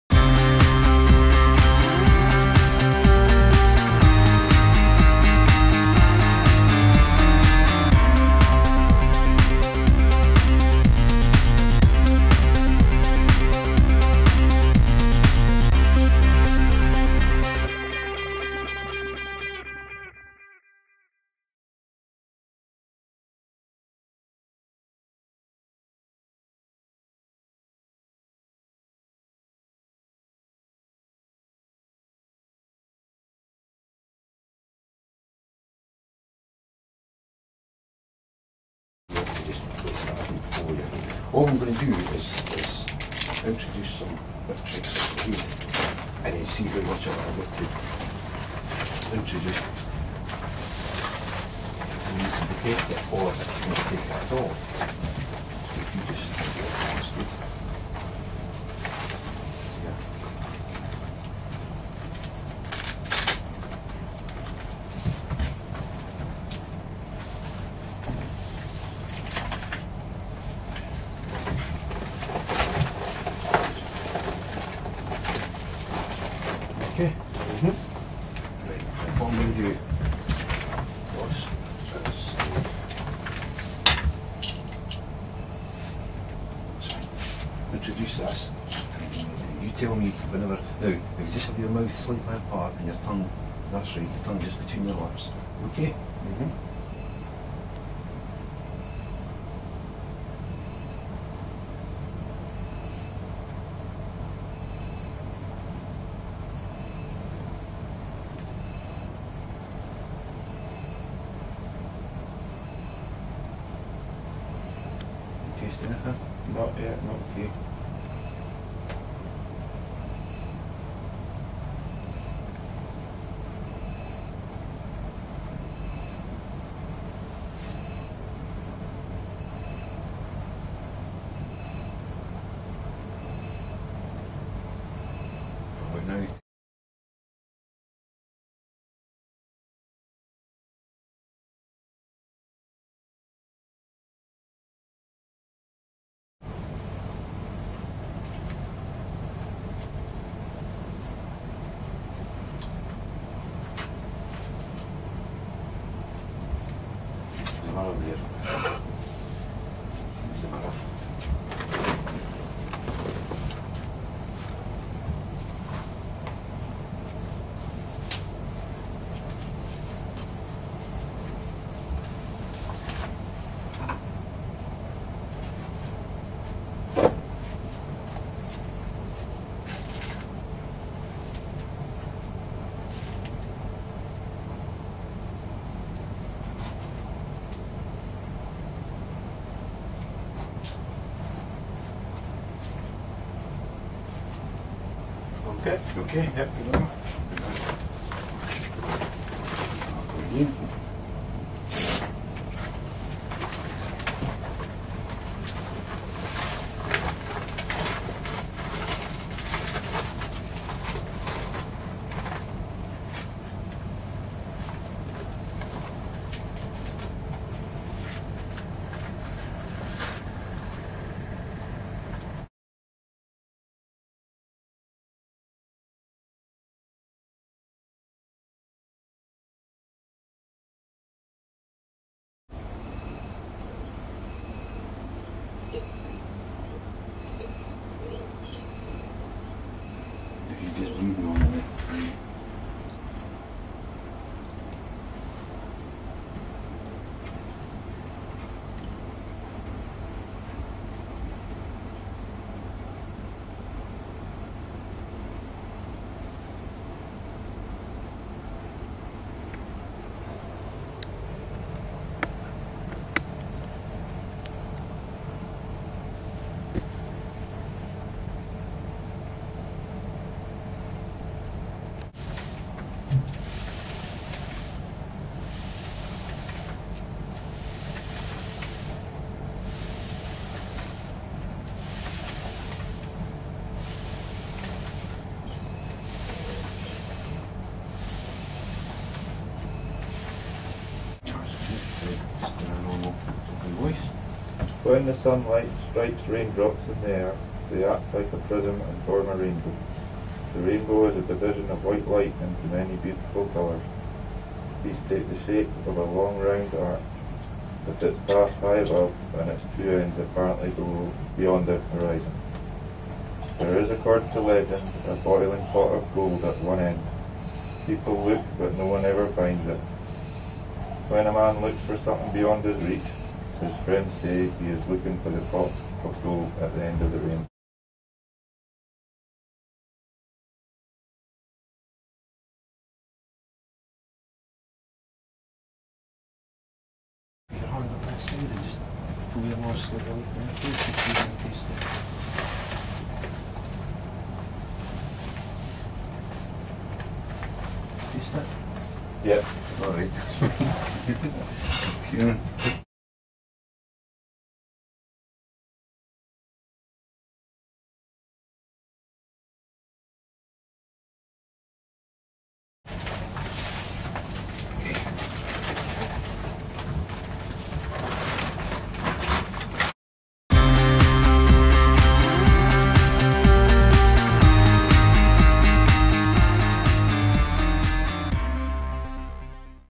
Qualitative RPE testing
This is a short introduction to qualitative fit testing of respirators that is intended for use in vocational training of apprentice stonemasons, MVR repair technicians and welders.